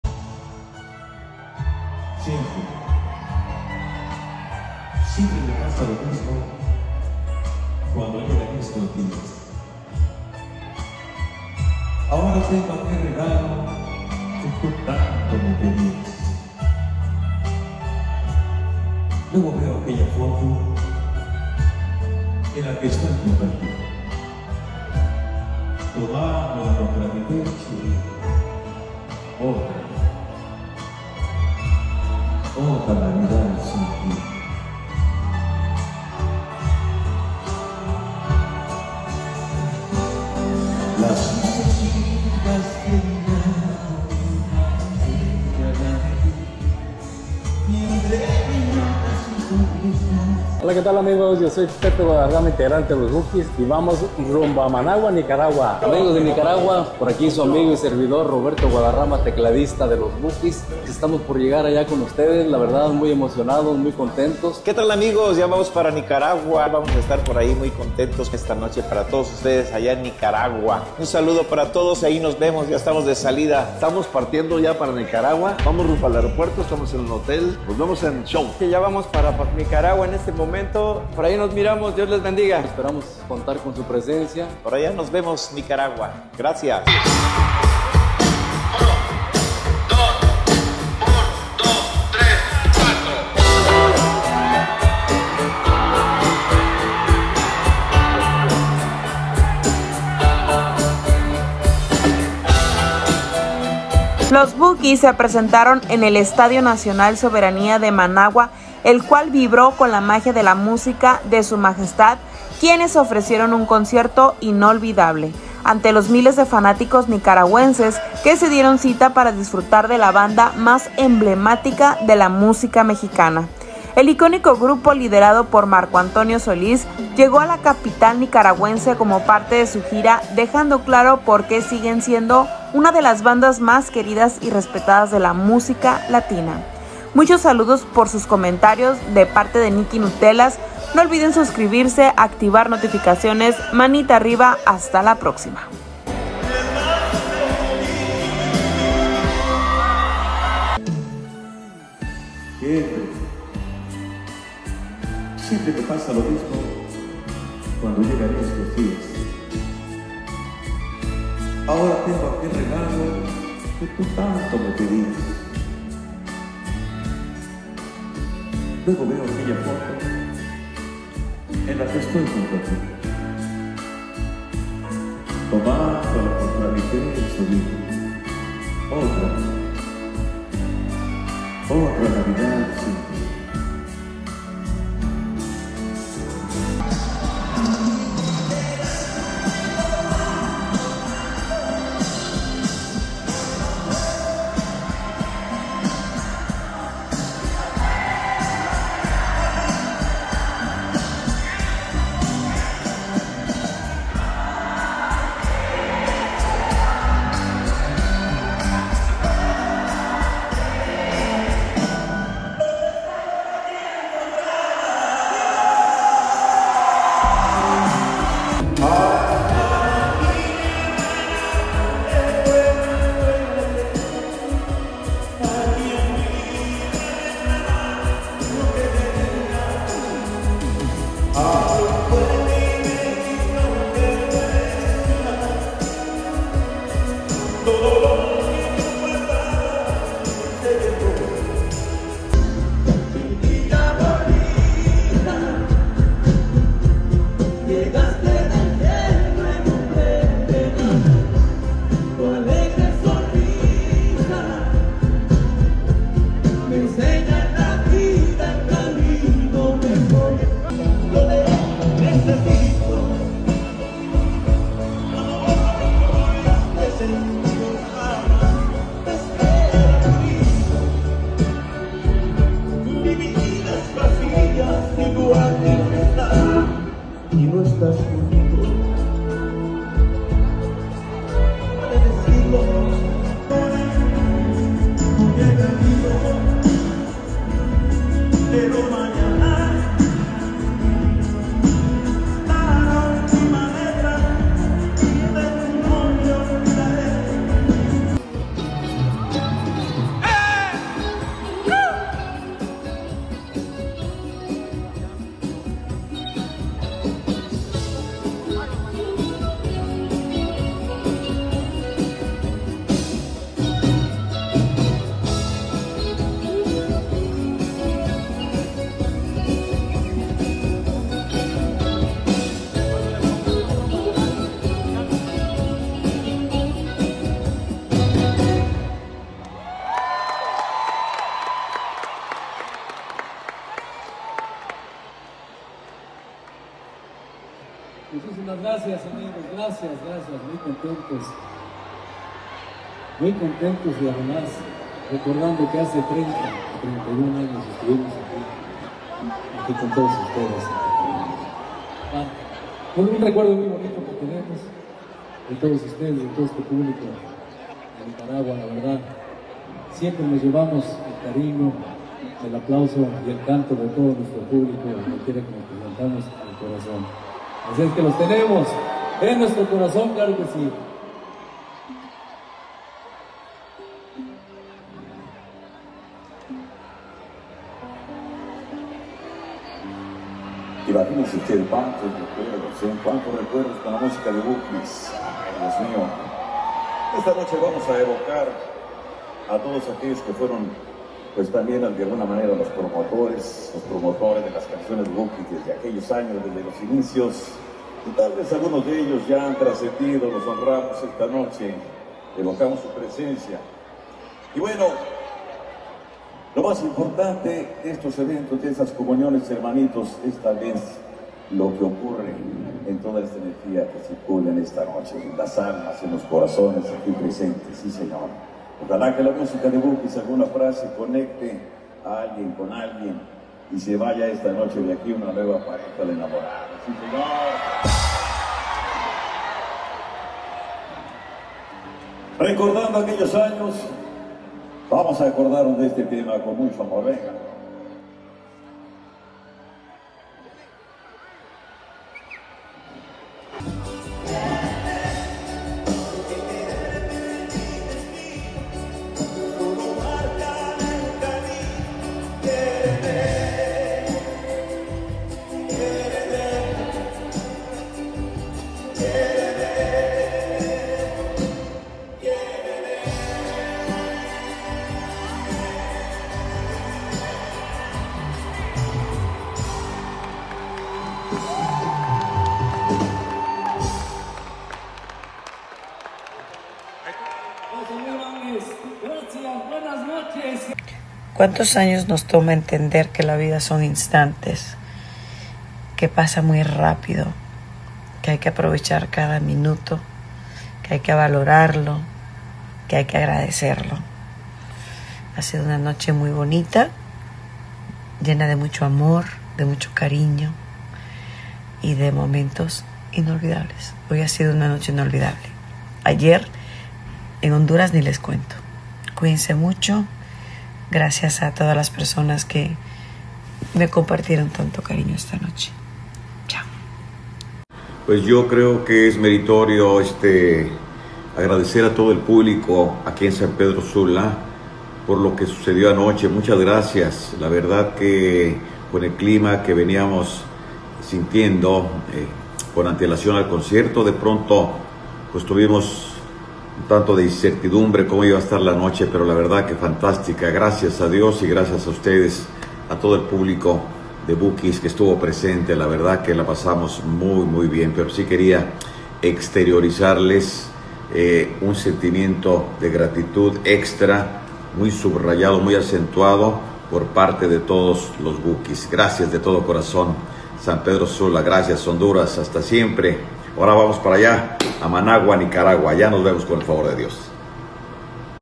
"Los Bukis Cautivan a Nicaragua ¡Un Show Que Jamás Olvidarán!"
Nicaragua fue testigo de uno de los conciertos más esperados y vibrantes de su historia reciente: la presentación de Los Bukis en un evento que marcó un antes y un después en el panorama musical del país. La legendaria banda mexicana, liderada por el icónico Marco Antonio Solís, llegó a Managua para hacer vibrar a miles de fans con su inconfundible estilo y una energía que desbordó el escenario.

"Los Bukis Arrasan en Honduras: ¡Un Show Épico que No Te Puedes Perder!"

Muchos saludos y bendiciones donde quiera que estés, Gracias por visitar mi canal😍💞